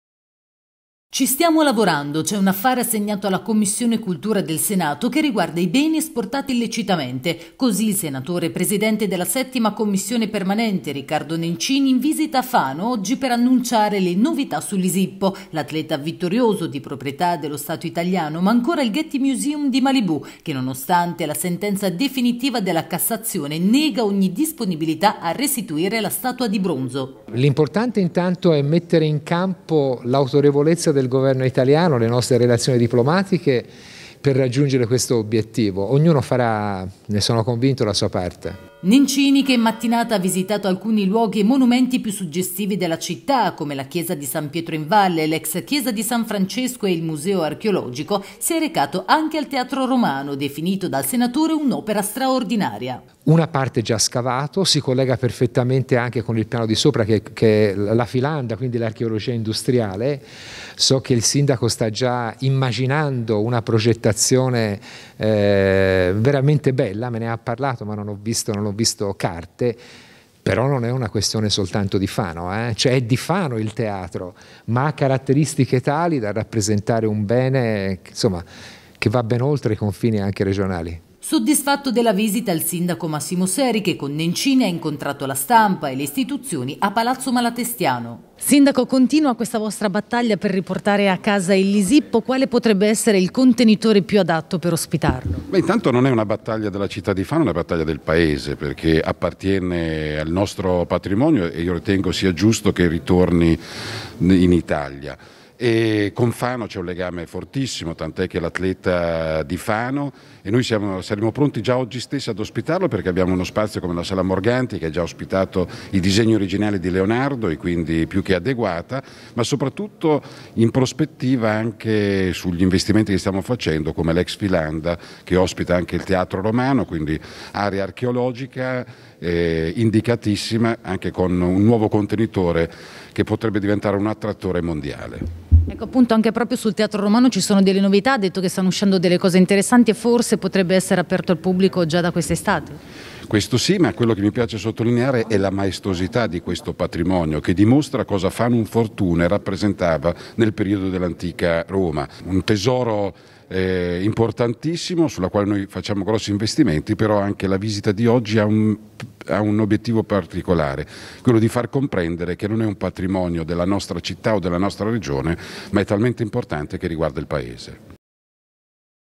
Nencini a Fano per Lisippo e teatro romano
“Siamo al lavoro per riportare a casa il Lisippo”. Lo ha assicurato oggi Riccardo Nencini, presidente della Commissione dei Beni Culturali, in visita alla città di Fano.